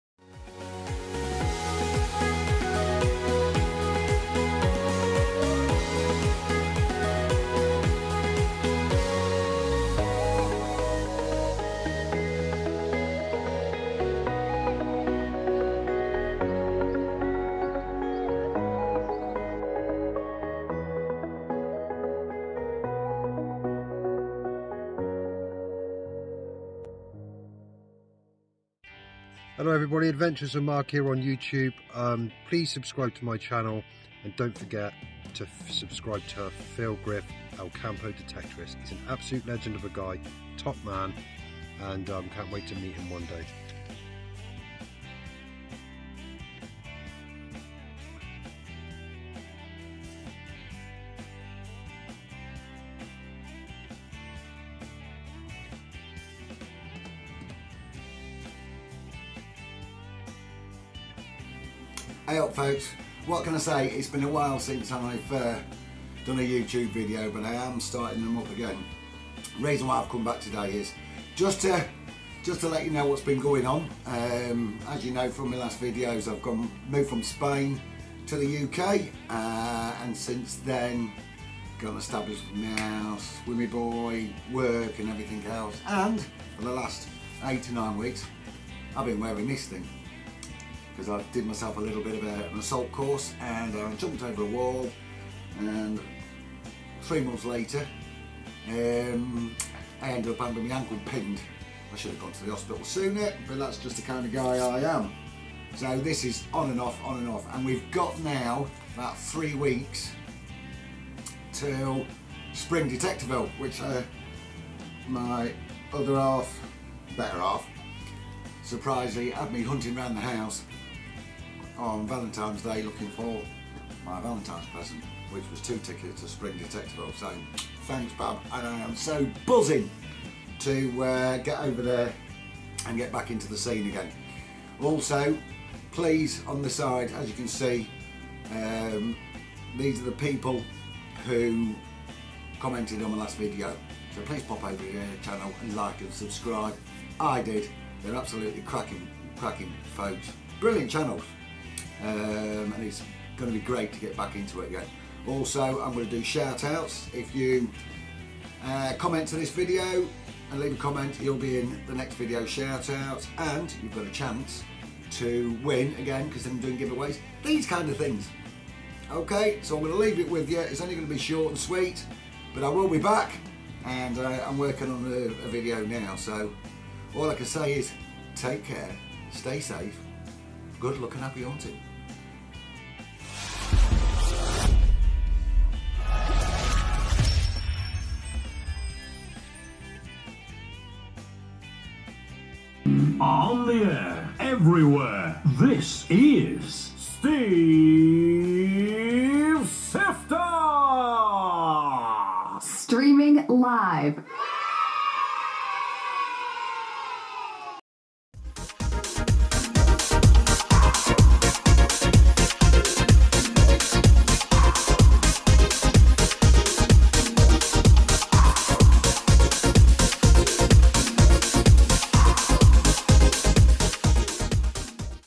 Back with more videos, Shout-outs & Subscribers Giveaways.
this video's just to tell you all I'm back on YouTube, with all different kinds of videos/episodes, from road-trips to metal detecting...
hope you enjoy, thank you for your support it's always appreciated..
take care, Stay Safe, Good Luck & Happy Hunting